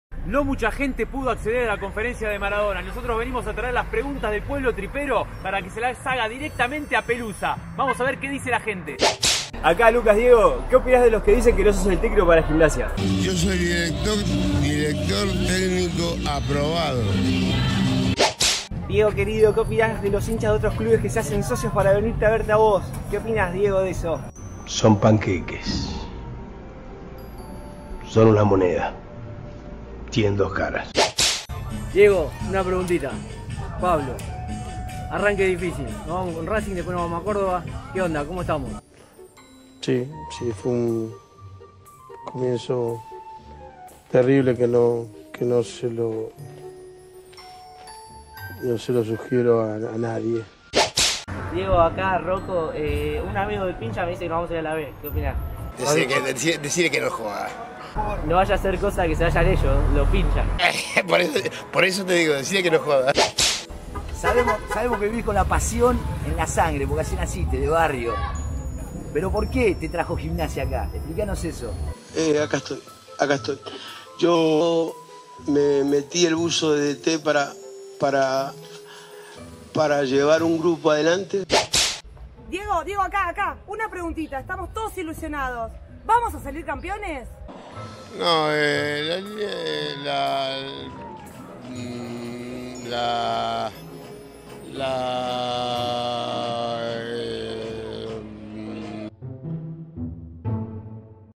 Conferencia inédita de Maradona
Los hinchas triperos le hicieron varias preguntas al Diego que no salieron en ningún medio. En Un Metro Adelantado TV vas a poder ver a Maradona contestando a calzón quitado. Compartí este material con tus amigos y seguinos en nuestro canal de YouTube.